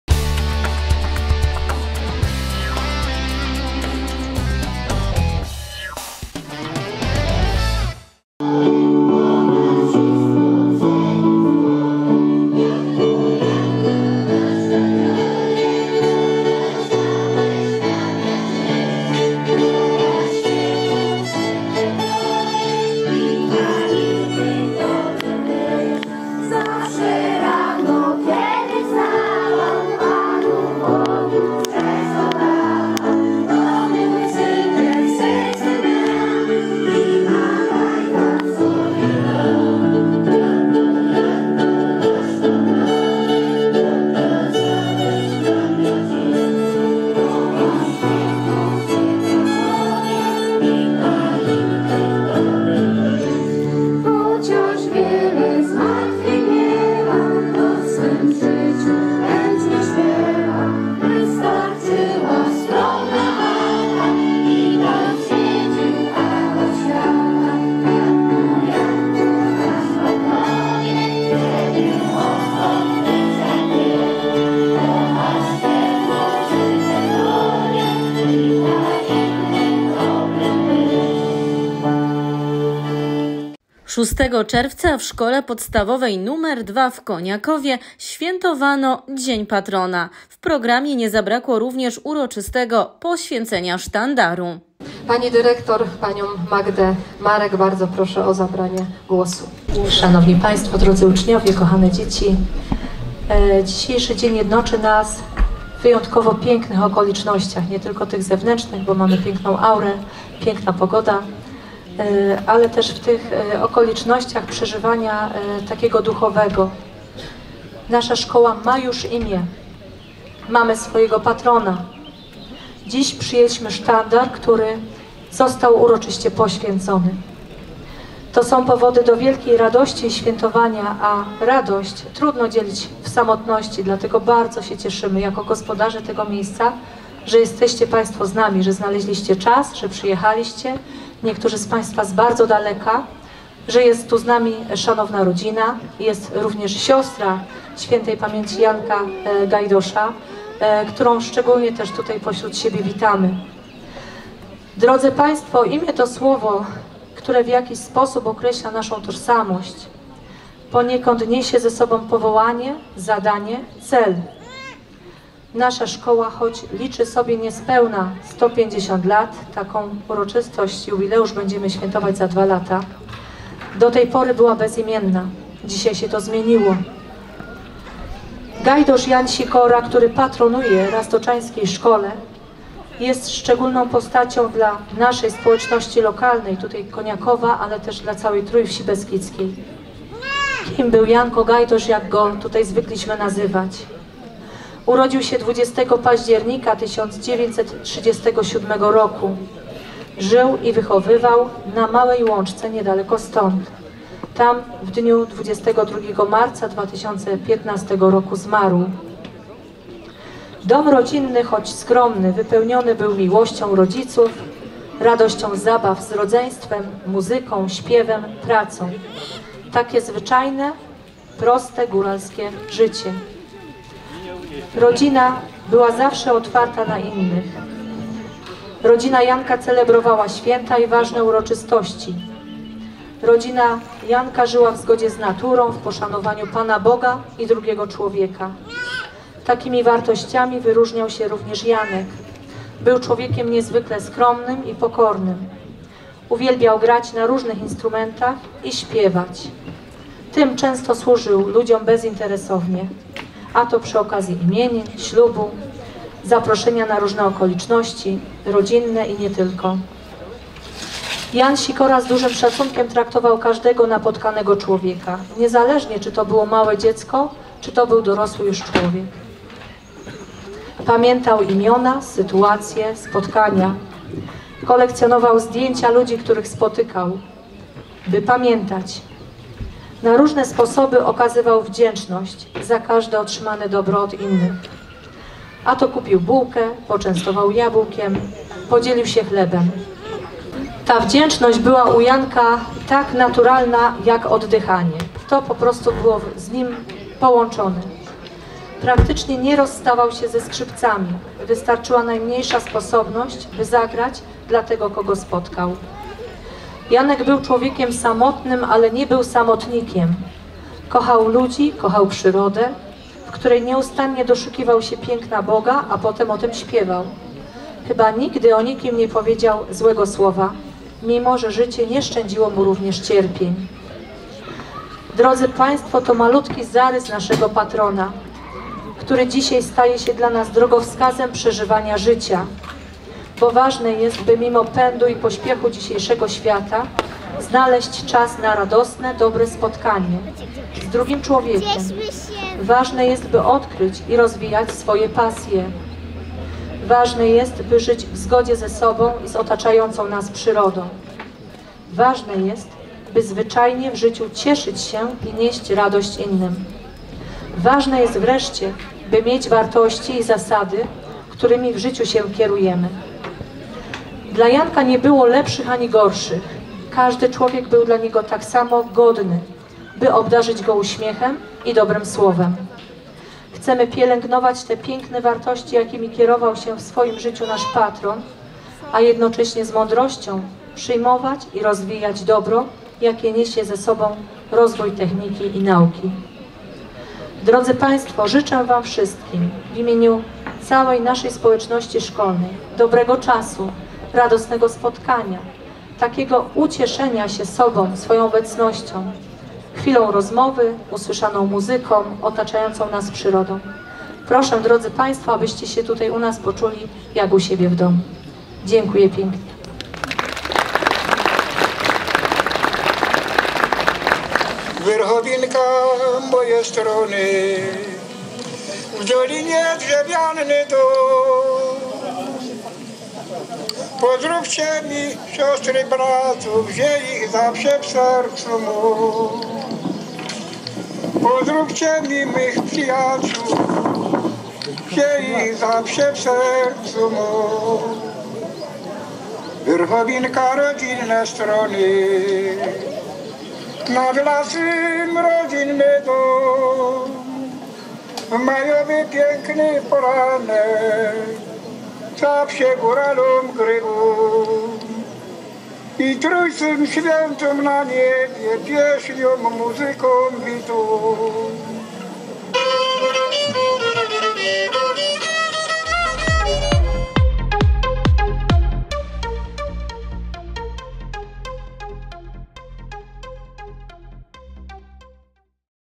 Uroczystość pierwszych obchodów Dnia Patrona i poświęcenia Sztandaru Szkoły
W czwartek (06.06) na terenie Szkoły Podstawowej nr 2 w Koniakowie im. Gajdosza Jana Sikory miała miejsce uroczystość pierwszych obchodów Dnia Patrona i  poświęcenia Sztandaru Szkoły. Wydarzenie  miało charakter plenerowy.